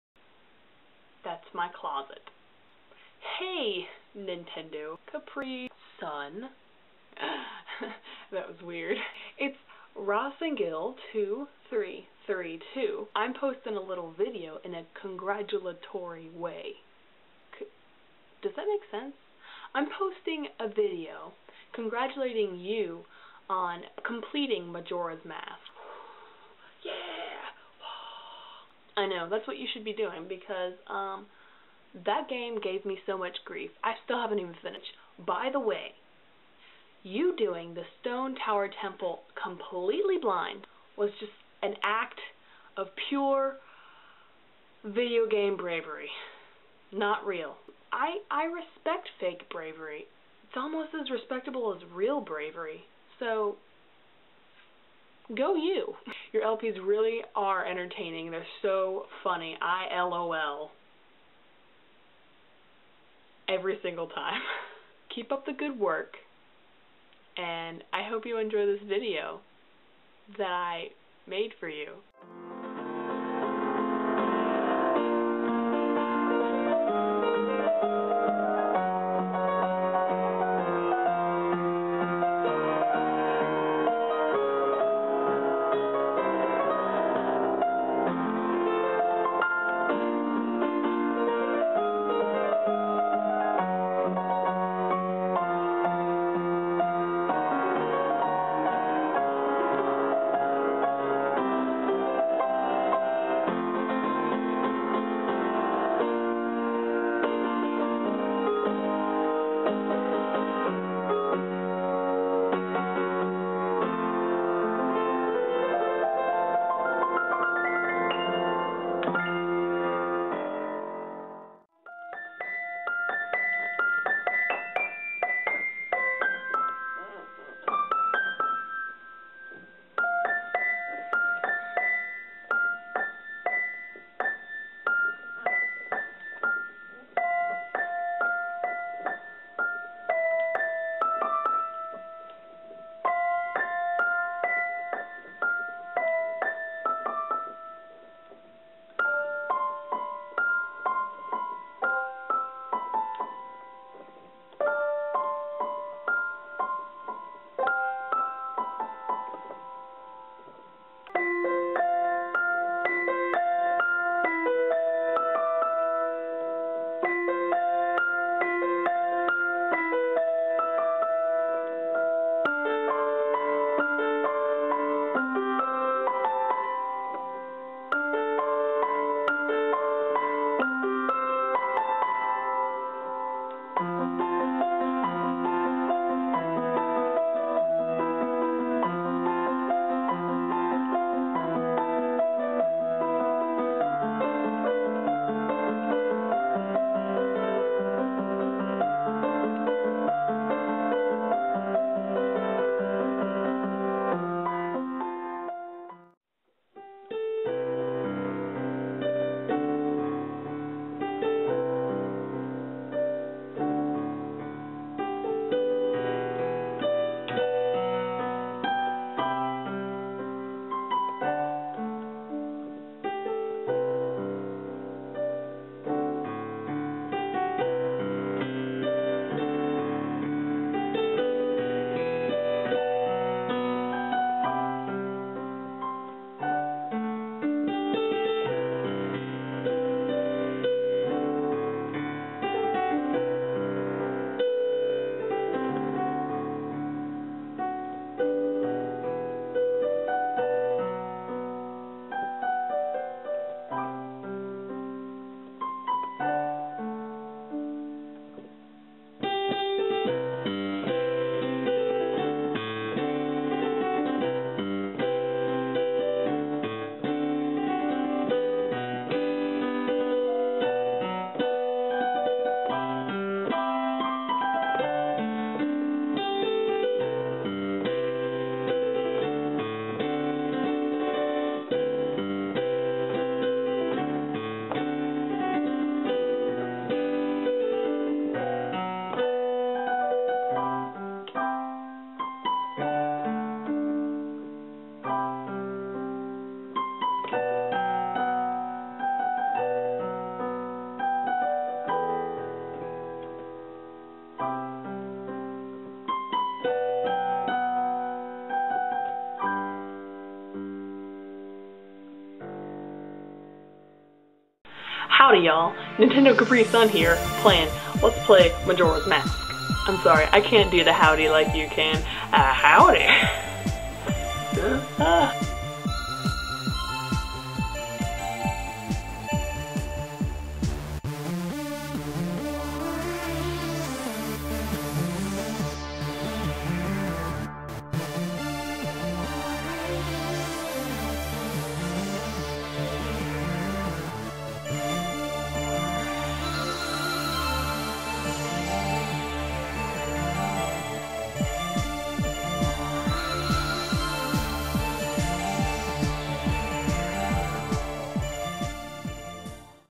A present for YOU!
I came up with this stuff pretty quick so its not very pretty. Just a way to show my appriciation to NintendoCapriSun for all the laughs. Keep doin whatcha doin and I'm excited for LPs the future holds!